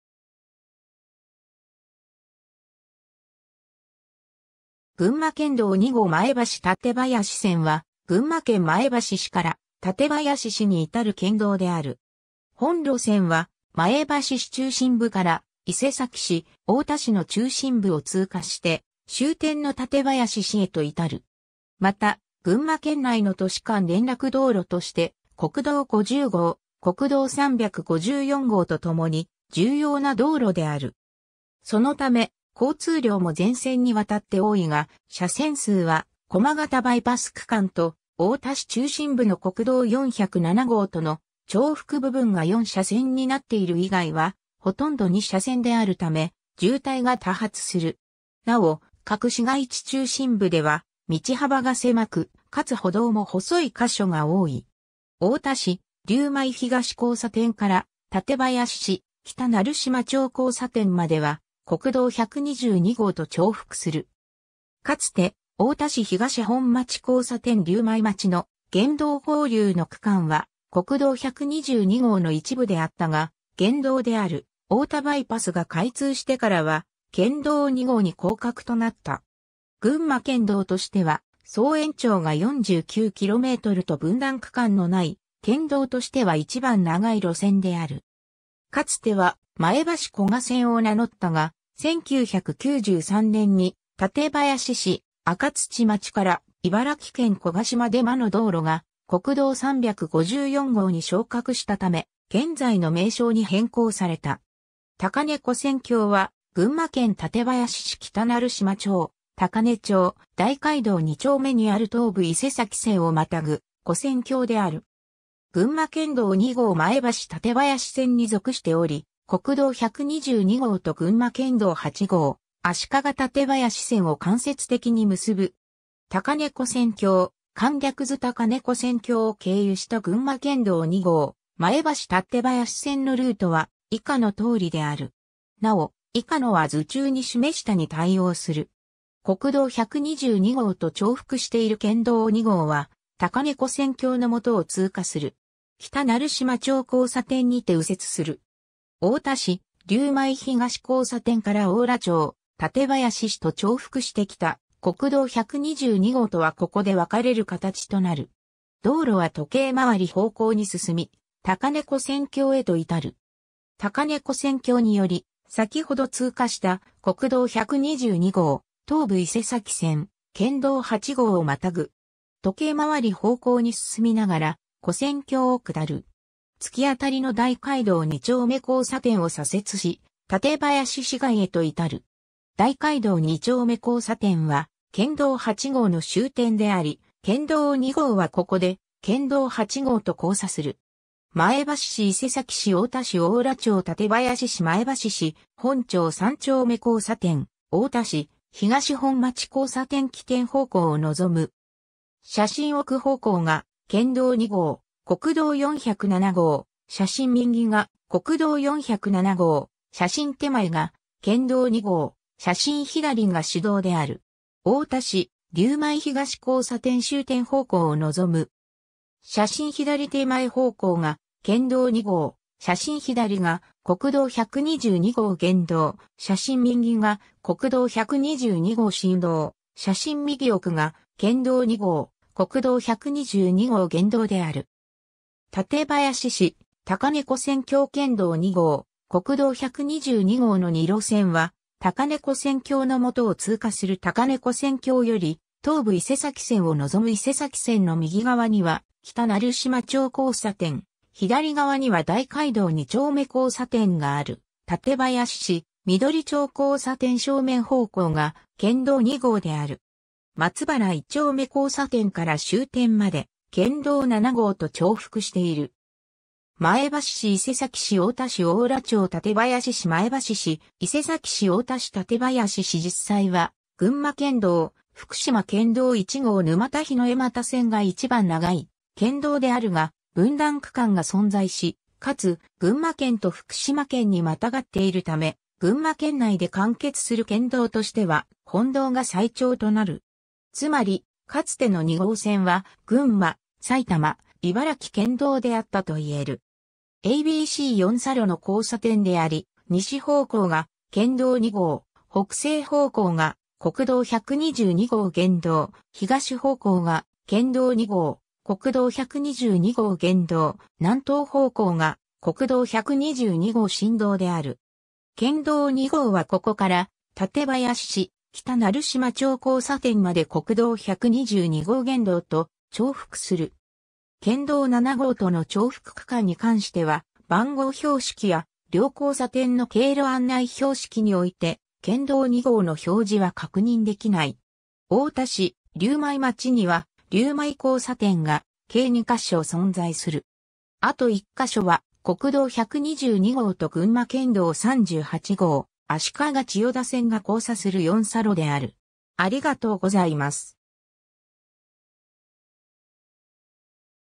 群馬県道2号前橋館林線
群馬県道2号前橋館林線（ぐんまけんどう2ごう まえばしたてばやしせん）は、群馬県前橋市から館林市に至る県道（主要地方道）である。
 本路線は、前橋市中心部から伊勢崎市、太田市の中心部を通過して終点の館林市へと至る。
また群馬県内の都市間連絡道路として国道50号、国道354号とともに重要な道路である。
そのため交通量も全線に渡って多いが、車線数は駒形バイパス区間と太田市中心部の国道407号との重複部分が4車線になっている以外はほとんど2車線であるため、渋滞が多発する。
なお各市街地中心部では道幅が狭く、かつ歩道も細い箇所が多い。
 太田市・竜舞東交差点から館林市・北成島町交差点までは、国道122号と重複する。
かつて太田市東本町交差点 - 竜舞町の現道合流（竜舞東交差点付近）の区間は国道122号の一部であったが、現道である太田バイパスが開通してからは県道2号に降格となった。
 群馬県道としては総延長が49 kmと分断区間のない県道としては1番長い路線である。
 かつては前橋古河線を名乗ったが、1993年（平成5年）に館林市赤土町から茨城県古河市まで間の道路が国道354号に昇格したため、現在の名称に変更された。
 高根跨線橋は、群馬県館林市北成島町/高根町/大街道2丁目にある東武伊勢崎線をまたぐ跨線橋である。
群馬県道2号前橋館林線に属しており、国道122号と群馬県道8号足利館林線を間接的に結ぶ。
 高根跨線橋・簡略図（2015年7月作成） 高根跨線橋を経由した群馬県道2号前橋館林線のルート（起点→終点方向）は以下のとおりである。
なお以下の (1)〜 (6) は図中に示した (1)〜 (6) に対応する。
 国道122号と重複している県道2号は、高根跨線橋の下を通過する。
 北成島町交差点にて右折する。
太田市・竜舞東交差点から邑楽町、館林市と重複してきた国道122号とはここで別れるかたちとなる。
 道路は時計回り方向に進み、高根跨線橋へと至る。
 高根跨線橋により先ほど通過した国道122号（県道2号が重複）、東武伊勢崎線、県道8号をまたぐ。
 時計回り方向に進みながら跨線橋を下る。
 突き当りの大街道二丁目交差点を左折し、館林市街へと至る。
大街道二丁目交差点は県道8号の終点であり、県道2号はここで県道8号と交差する。
 前橋市 伊勢崎市 太田市 邑楽町 館林市 前橋市・本町三丁目交差点 （起点、2015年8月） 太田市・東本町交差点 起点方向を望む。
写真奥（西）方向が県道2号（至伊勢崎市）・国道407号、写真右（北）が国道407号、写真手前（東）が県道2号（至邑楽町）、写真左（南）が市道である。
 太田市・竜舞東交差点 （龍舞町、2015年7月） 終点方向を望む。
写真左手前方向が県道2号、写真左が国道122号現道、写真右が国道122号新道、写真右奥が県道2号・国道122号現道である。
 館林市・高根跨線橋 （北成島町、2015年6月） 県道2号・国道122号の2路線は高根跨線橋（県道2号）の下を通過する 高根跨線橋より東武伊勢崎線（南東方向）を望む（2015年6月） 伊勢崎線の右側には北成島町交差点（国道122号と交差）、左側には大街道二丁目交差点（県道8号と交差）がある 館林市・緑町交差点 （終点、2015年6月） 正面方向が県道2号である。
松原一丁目交差点から終点まで県道7号と重複している。
 前橋市 伊勢崎市 太田市 邑楽町 館林市 前橋市 伊勢崎市 太田市 館林市 ^ 実際は群馬県道・福島県道1号沼田檜枝岐線が一番長い県道であるが、分断区間が存在し、かつ群馬県と福島県に跨っているため、群馬県内で完結する県道としては本道が最長となる。
 ^ つまり、かつての2号線は群馬・埼玉（加須市（旧北川辺町））・茨城（古河市）県道であったといえる。
 ^ a b c 四差路の交差点であり、西（写真左手前）方向が県道2号（東国文化歴史街道、至伊勢崎市）、北西（写真左）方向が国道122号現道（太田バイパス、至桐生市）、東（写真右奥）方向が県道2号・国道122号現道（東国文化歴史街道、至邑楽町）、南東（写真右）方向が国道122号新道（八重笠道路、至大泉町）である。
県道2号はここから館林市・北成島町交差点まで国道122号現道と重複する。
 ^ 県道7号との重複区間に関しては、番号標識や両交差点の経路案内標識において県道2号の表示は確認できない（県道7号のみが表示されている）。
 ^ 太田市龍舞町には「竜舞交差点」が計2か所存在する。
あと1か所は国道12...